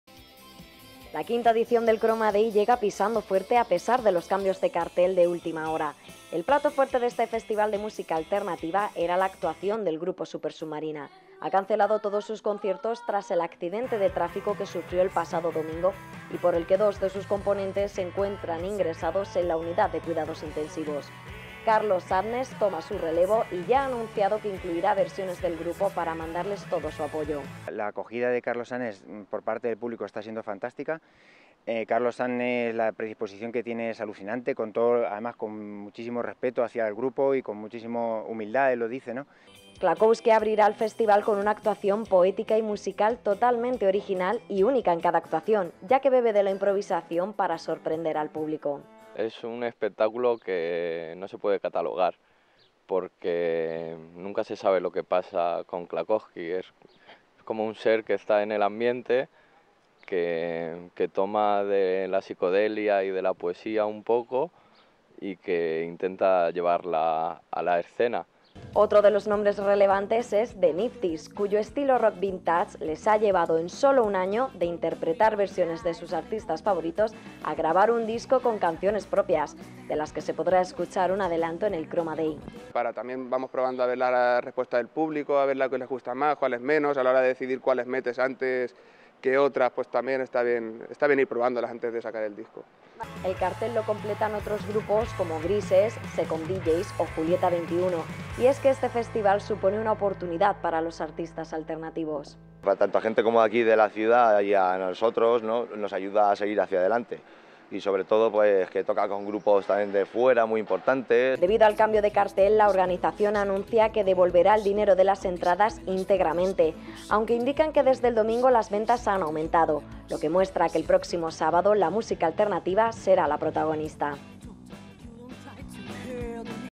CUENTA ATRÁS PARA EL FESTIVAL CROMA DAY DE ALBACETE
El festival de música alternativa ‘Croma Day’ cuenta ya con nueva cabeza de cartel. Carlos Sadness será el sustituto del grupo Supersubmarina, que ha cancelado todas sus actuaciones tras el accidente de tráfico que sufrió el pasado domingo. Tendrá lugar el próximo sábado en la caseta de los Jardinillos de Albacete y contará con otros artistas como Grises, The Niftys o Clacousqui.